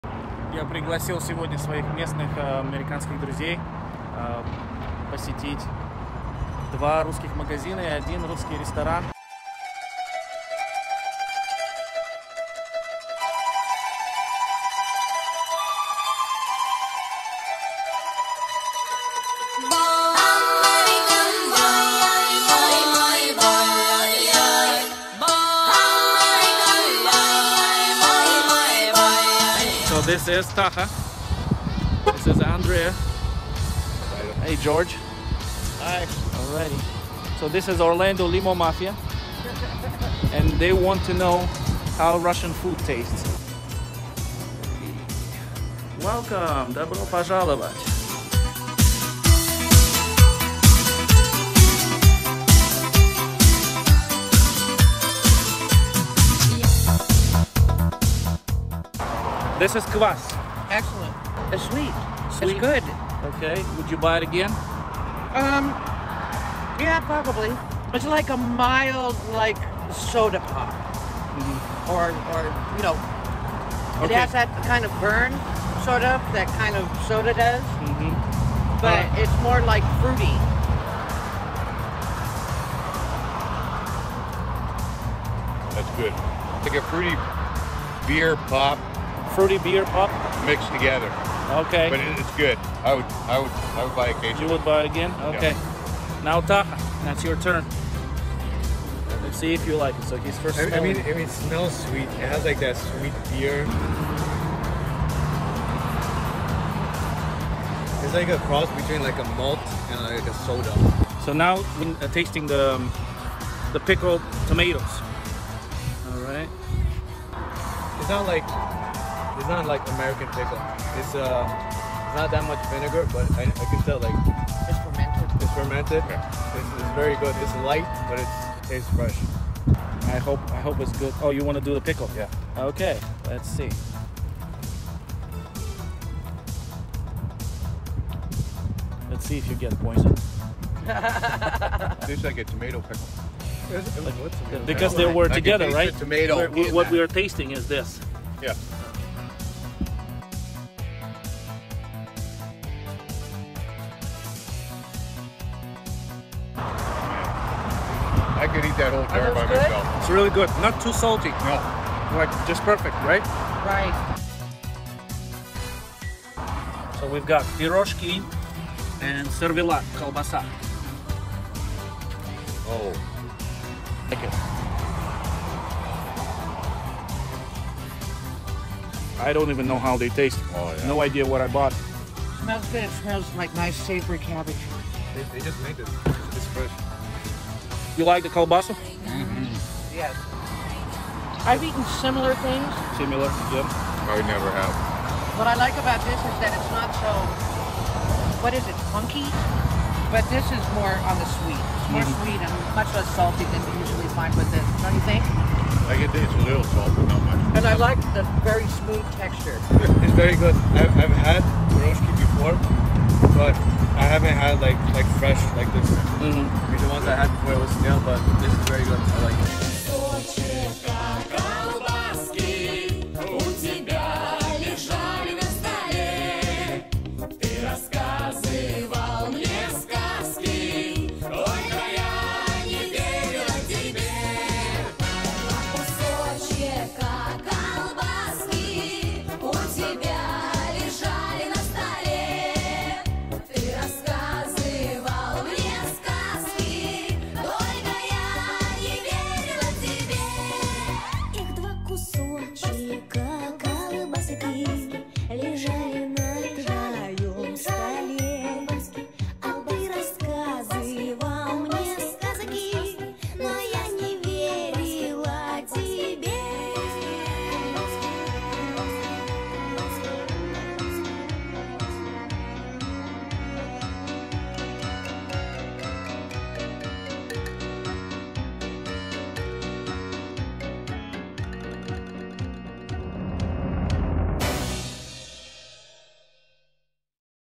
Russian food tour in Orlando, Florida
Americans taste Russian cuisine for the first time. Kvass, pelmeni, pierozhki, borscht, blini, rassolnik.

Вкусовые ощущения Американцев в Орландо, штат Флорида, США, которые впервые пробуют русскую кухню. Пельмени, борщ, рассольник, блины, квас, солености, колбаса.